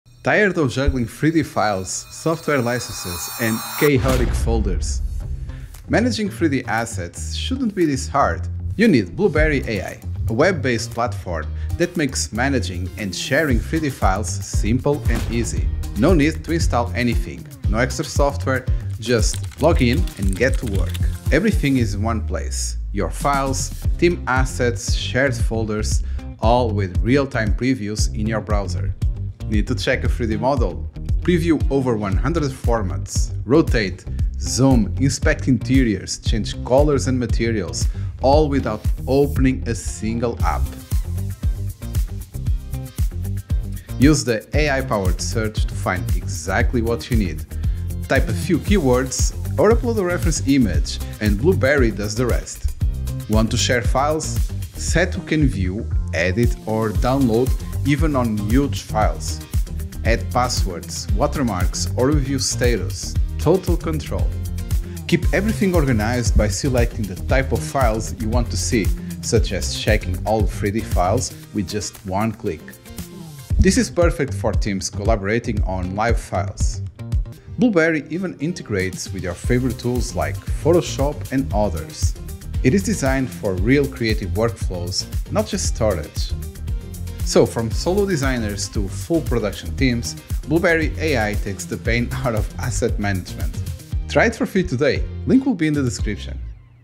I Tried the Future of 3D Asset Management — Blueberry AI TESTED
Stop wasting time with messy folders, endless software, and slow previews.
Blueberry AI is here to change how you manage and share your 3D files, no installs, no headaches.
Preview over 100 file formats in your browser, inspect models in real-time, and search files using AI.

🎯 Perfect for: 3D Artists, Archviz & Animation Studios, Game Devs &. Product Designers and Teams managing huge asset libraries

💡 Features:

- AI-powered search by keyword or image
- Real-time 3D preview (rotate, zoom, inspect interiors)
- Instant sharing with permission controls
- No software installs — all web-based
- Works with Photoshop and more